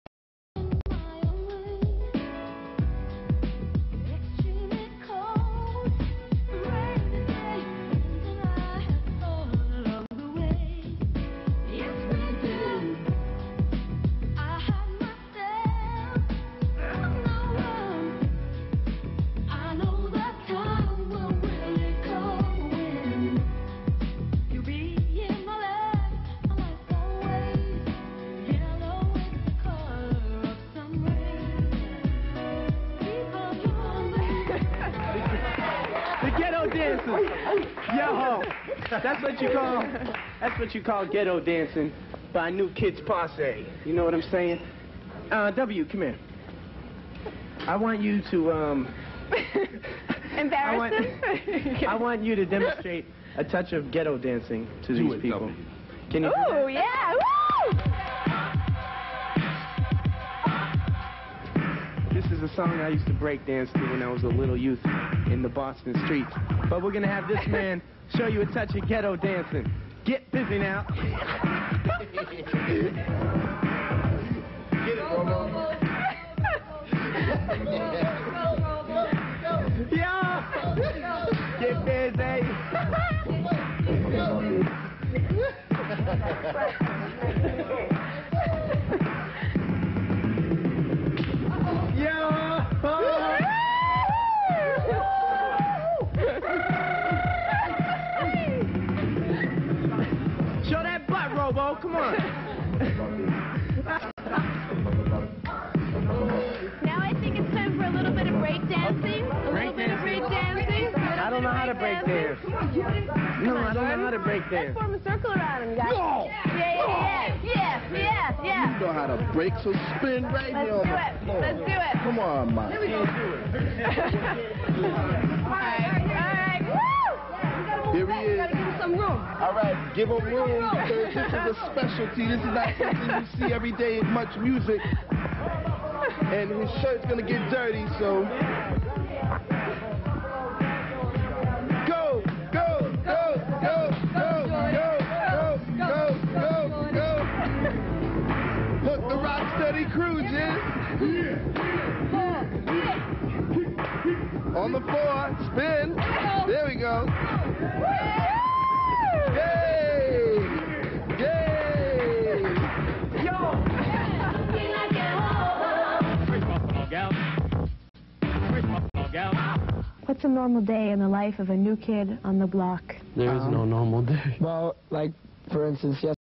NKOTB - Takin Over Canadian TV show - 1990Pt 6
Jordan doing the 'ghetto dance' :-)